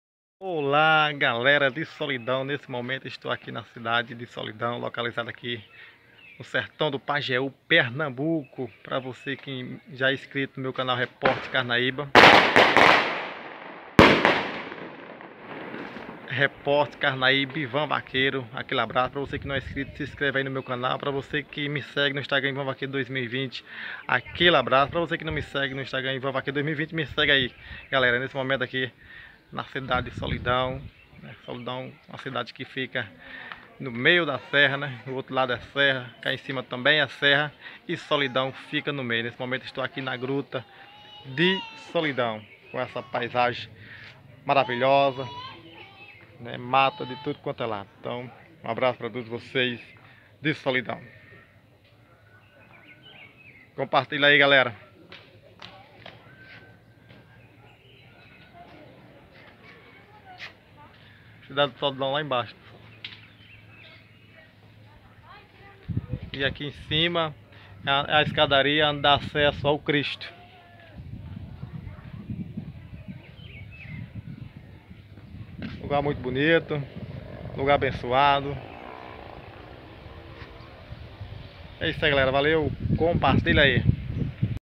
na cidade de solidão.
gruta de nossa senhora de Lourdes localizada na cidade de solidão Pernambuco.